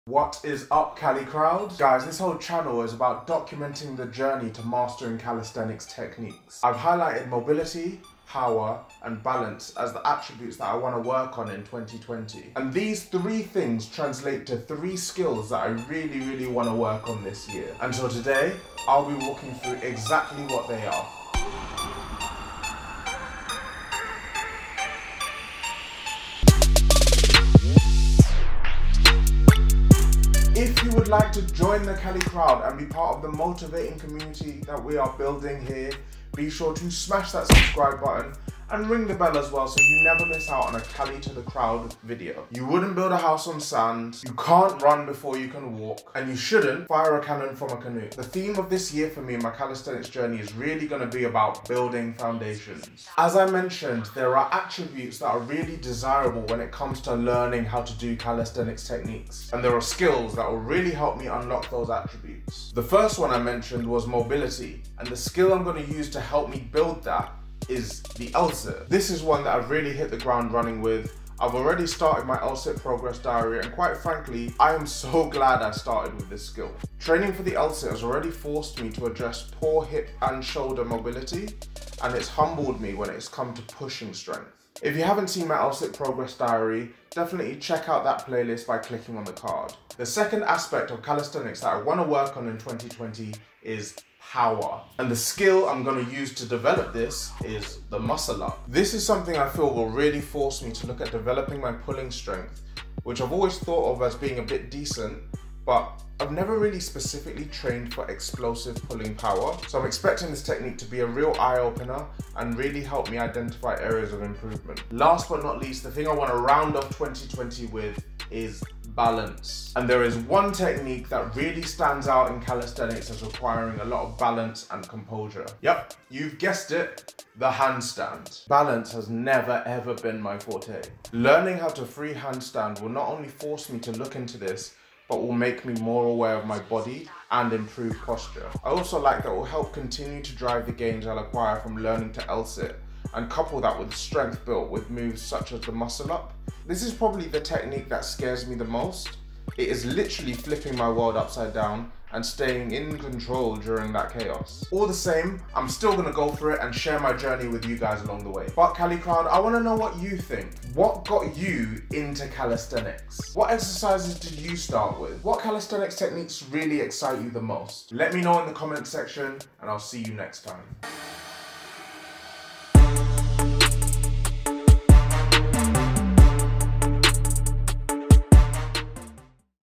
Calisthenics Goals - 2020 | Cali To The Crowd
2020 is all about building foundations that will serve over the long term when it comes to learning calisthenics techniques.

I have only just started training this way and I am already seeing benefits. And so in order to bring my aspirations into focus, this video highlights my plan for the year ahead.
Get the camouflage gear out and stalk me on Instagram:
Cali To The Crowd™ is built to help make calisthenics simple

mobility:  01:03
power: 01:30
balance: 02:00